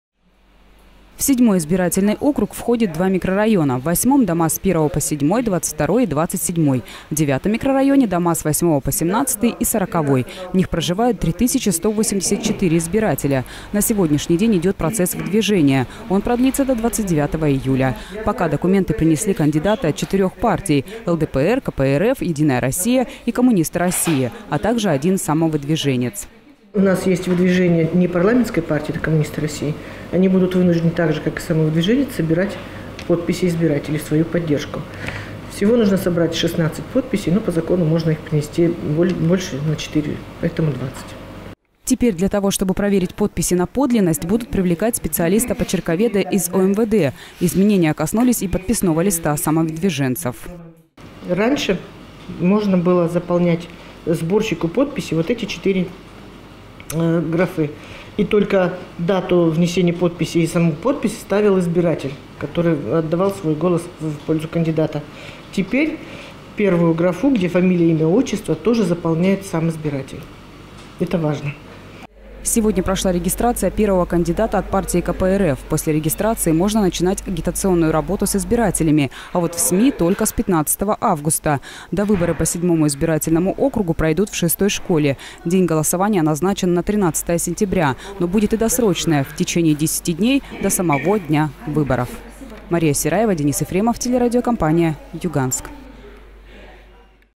Довыборы депутата
13 сентября в Нефтеюганске пройдут довыборы депутата 6 созыва по 7 одномандатному избирательному округу. Прием документов и регистрация кандидатов продлится до 29 июля. Сейчас на место народного избранника претендуют 5 человек.